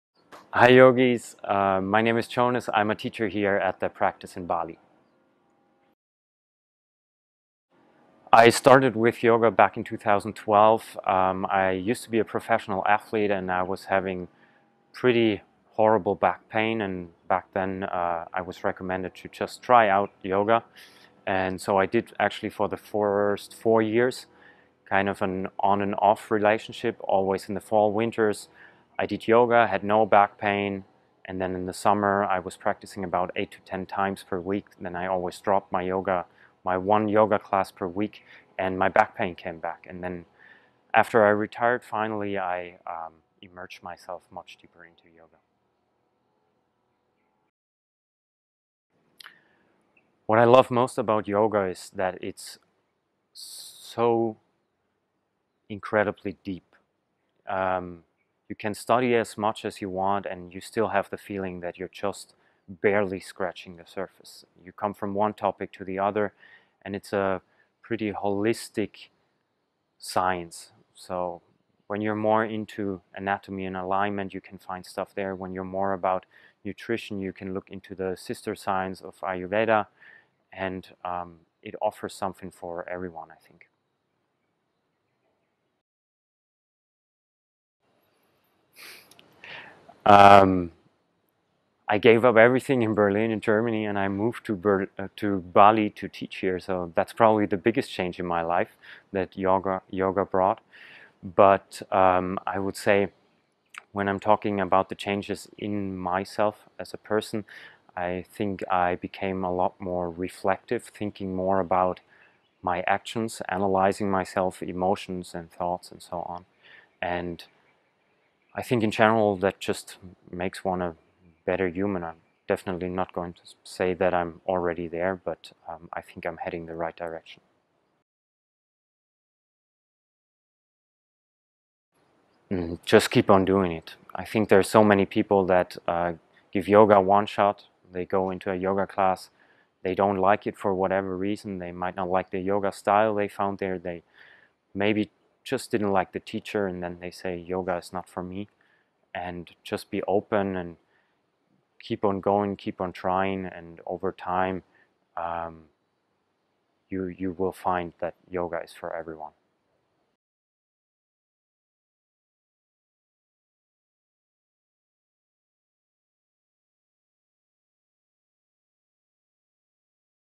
TEACHER INTERVIEWS │JONAS
Interview with Jonas Plass. Digging in deep and understanding his yoga journey from the get-go, along with some words of wisdom.

Get immediate access to a FREE 30-Day Trial of The Practice Online and evolve your practice: